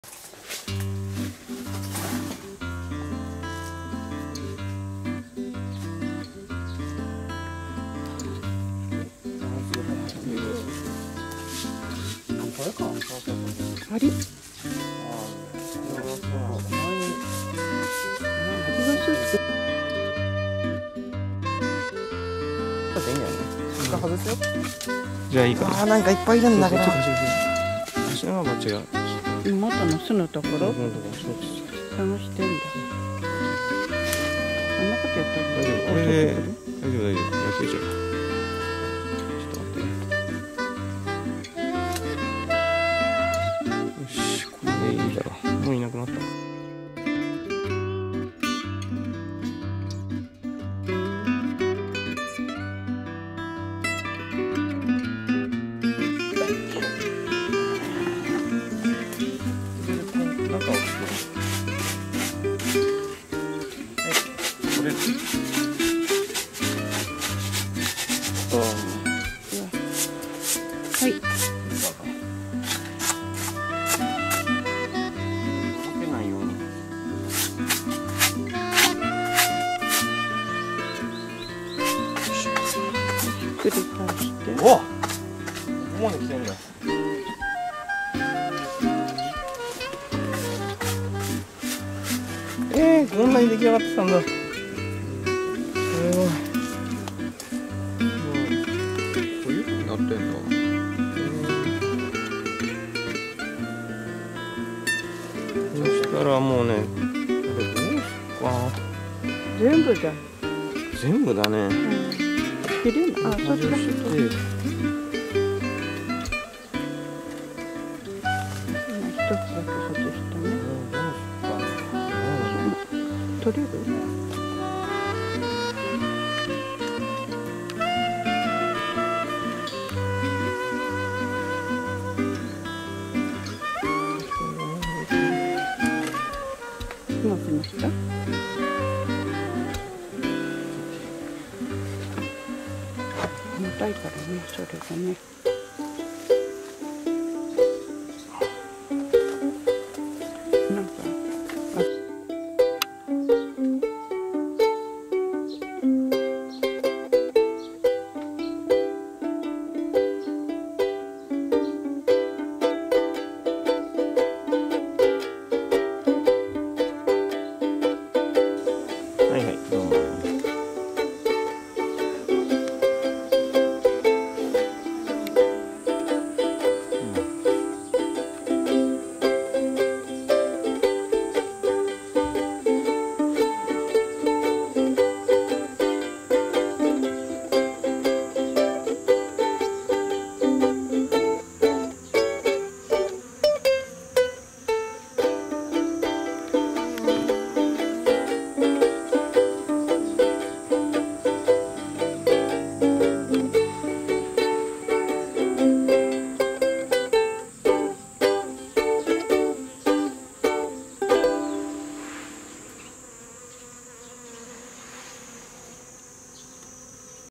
透明巣箱にミツバチ誘引
透明巣箱にミツバチを誘導します。巣箱を逆さまにしてその上に透明巣箱を設置、ミツバチが上に行くのを待ちます。上手くいくでしょうか(^_^;)　字幕で解説をしていますのでご覧ください。
作者の久住昌之さん、地底レコードの吉田さんより楽曲の使用許可をいただいております。